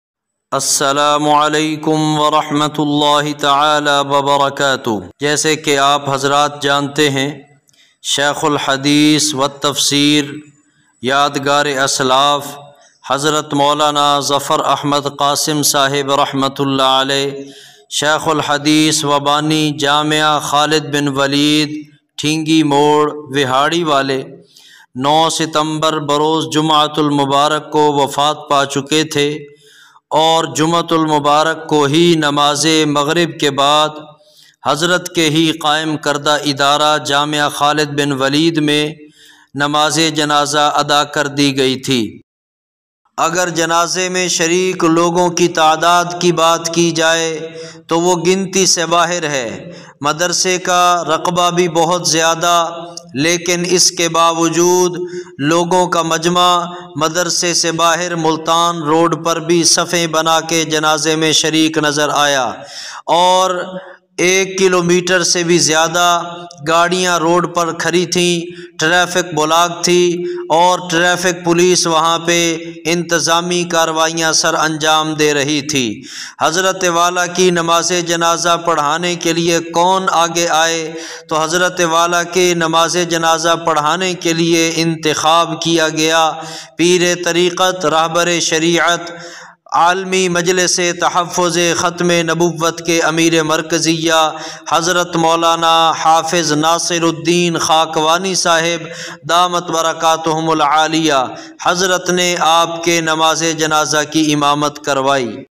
Molana Zafar Ahmad Qasim R.A ka Janaza kis ny parhaya |مولاناظفراحمدقاسم کاجنازہ کس نے پڑھایا ؟
شیخ الحدیث والتفسیر،حضرت مولاناظفراحمدقاسم رحمۃ اللّٰہ علیہ
کا جنازہ کس نے پڑھایا ؟
Jamia Khalid bin waleed Vehari
Molana Zafar Ahmad Qasim Funeral video